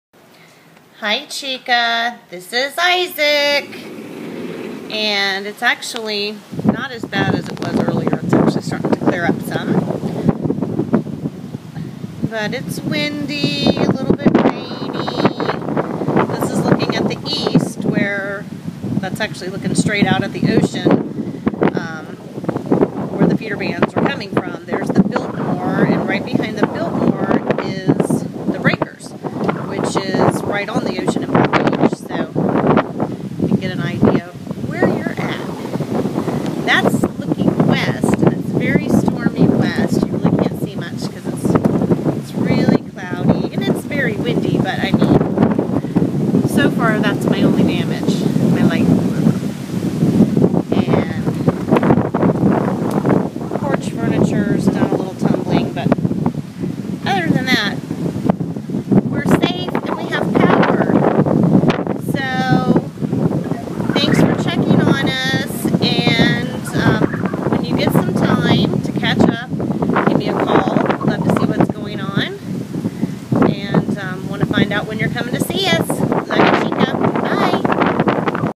Tropical Storm Isaac
For my friend Andi - a little break in between feeder bands coming in from the ocean side in downtown West Palm Beach.  Now the wind & rain are really slamming us again. :)